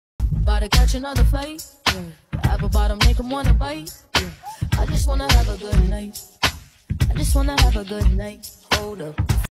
Louis Vuitton Monogram Papillon
Sharing my newest designer bag, the Louis Vuitton Monogram Papillion! The classic monogram print & unique shape make it a standout piece. Versatile for any occasion, I can't wait to style it in more videos!